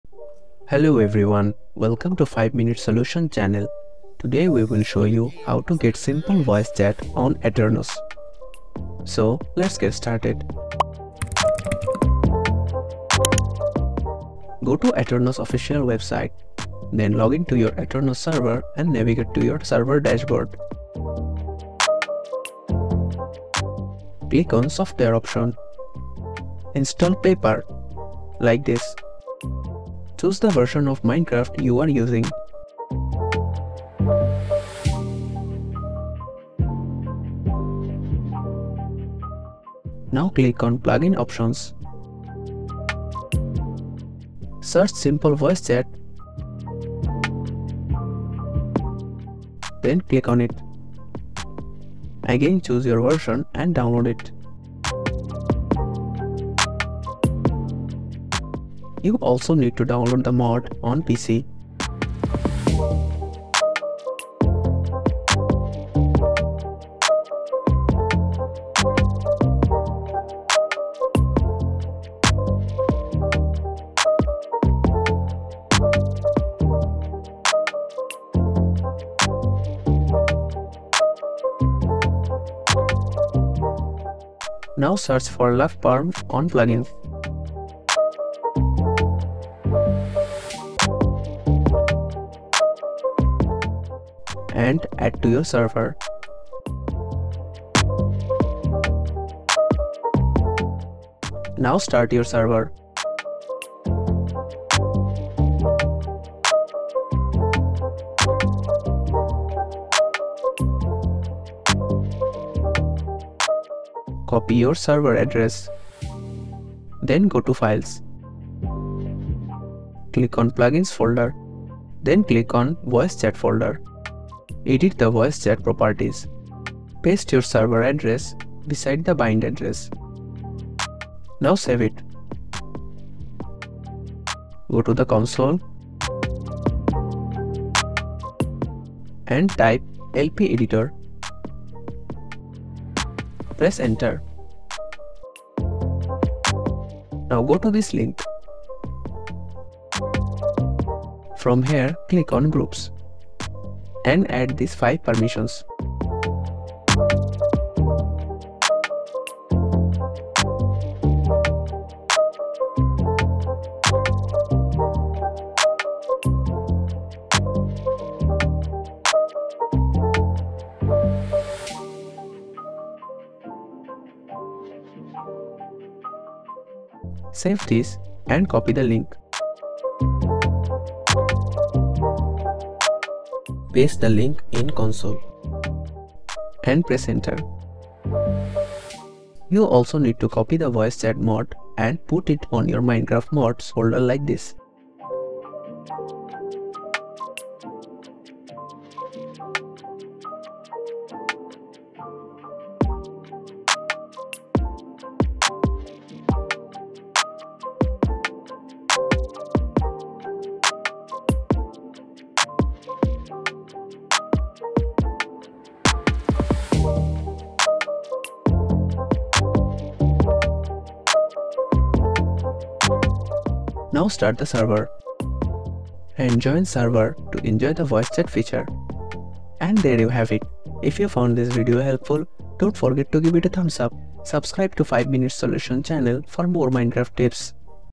How To Get Simple Voice Chat On Aternos - Full Guide
Want to Get Simple Voice Chat On Aternos, but can't find the right way? Don't worry. Here's all you need to do.

Video Parts:
0:00 Info & Intro
0:16 Go To Aternos official Website
0:25 Click on Software Options
0:41 Click on Plugin options
1:43 Start Server
2:59 Copy Link
3:51 Ending & Outro